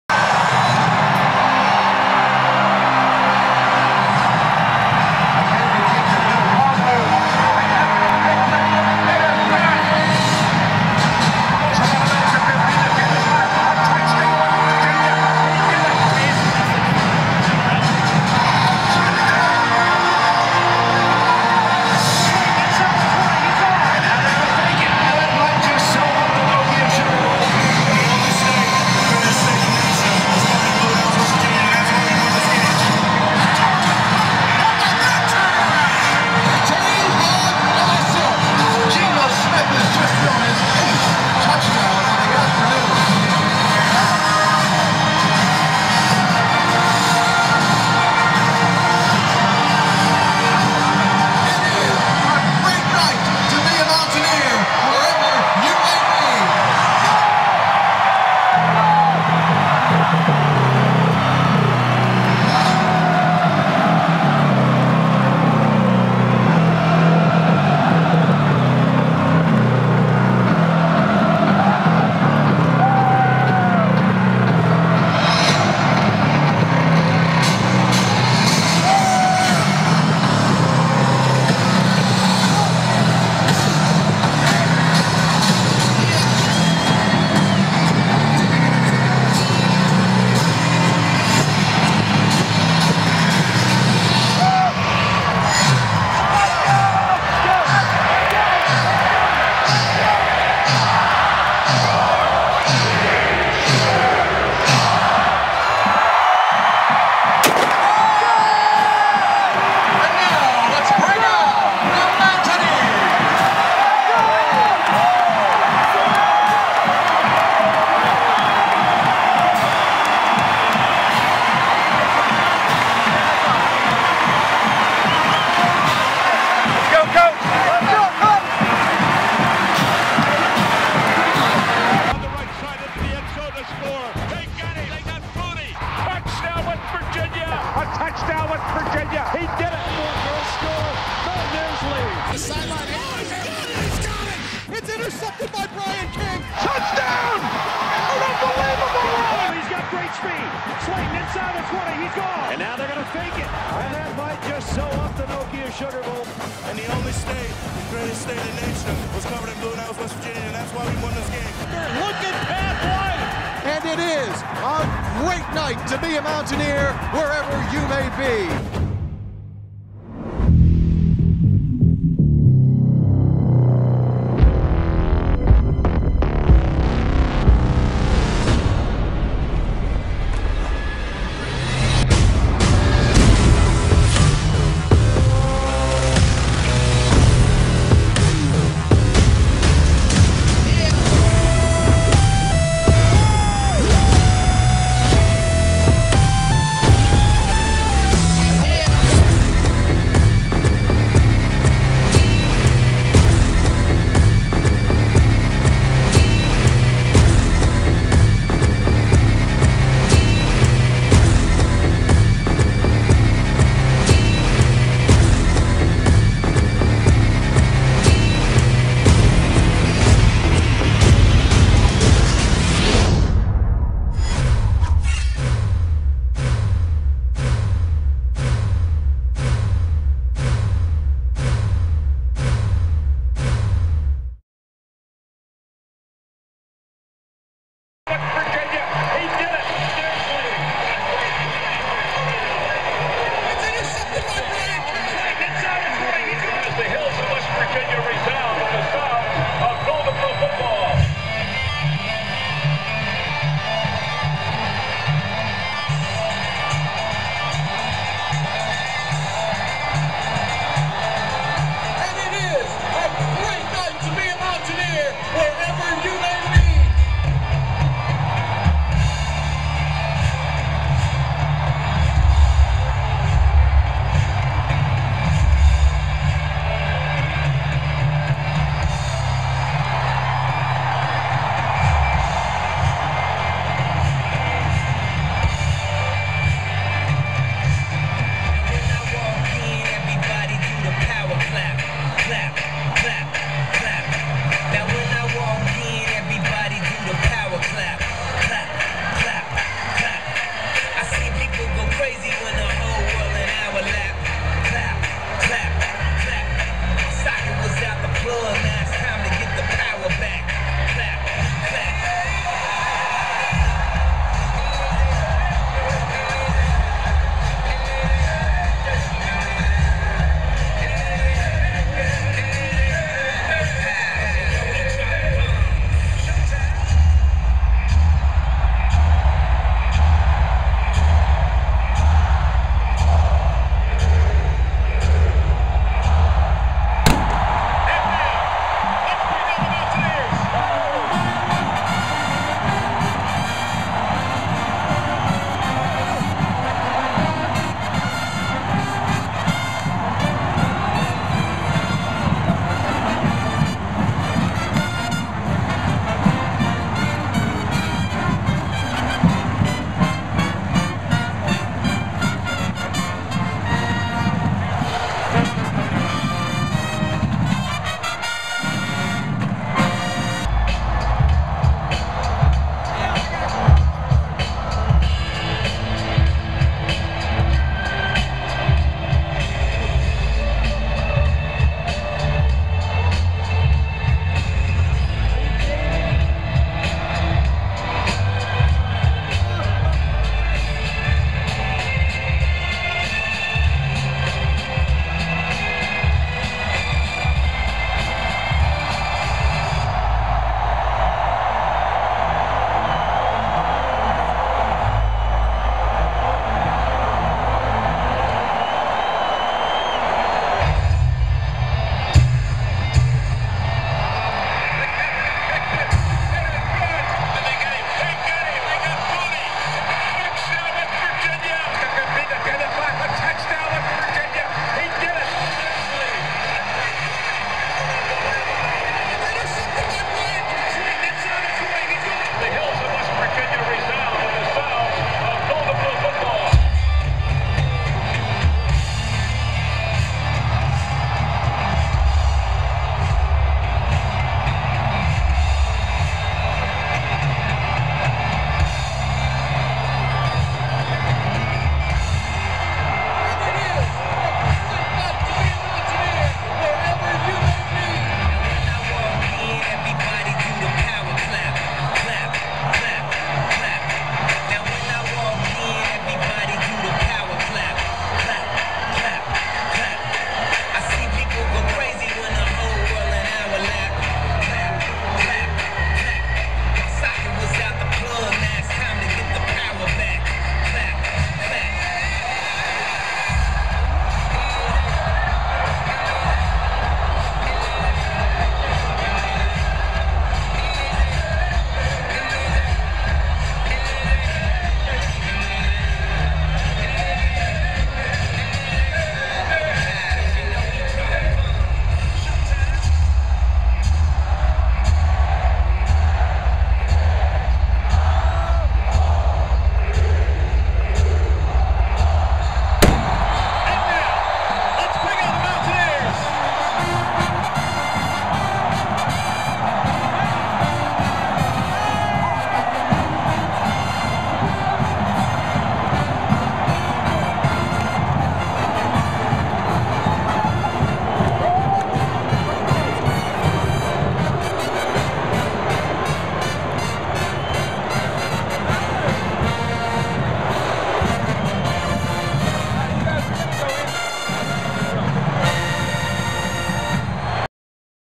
Wvu Football Entrance (2011/2013) Including Lsu
Here is a few of the many wvu football  entrances, This video does include the wvu vs lsu entrance.